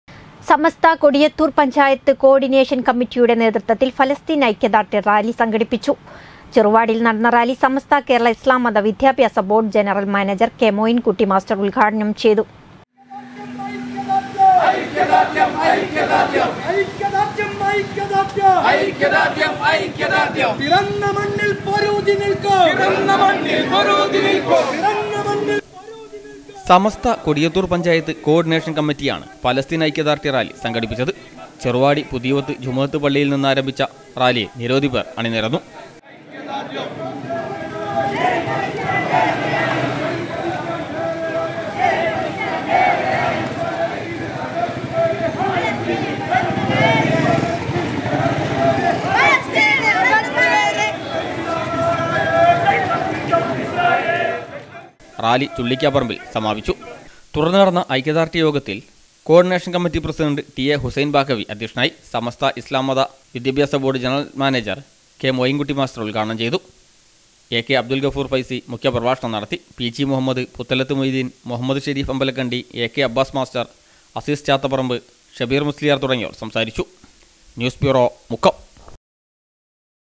കോ-ഓർഡിനേഷൻ കമ്മറ്റിയുടെ നേതൃത്വത്തിൽ ഫലസ്തീൻ ഐക്യദാർഡ്യ റാലി സംഘടിപ്പിച്ചു.
സമസ്ത കൊടിയത്തൂർ പഞ്ചായത്ത് കോ-ഓർഡിനേഷൻ കമ്മറ്റിയുടെ നേതൃത്വത്തിൽ ഫലസ്തീൻ ഐക്യദാർഡ്യ റാലി സംഘടിപ്പിച്ചു.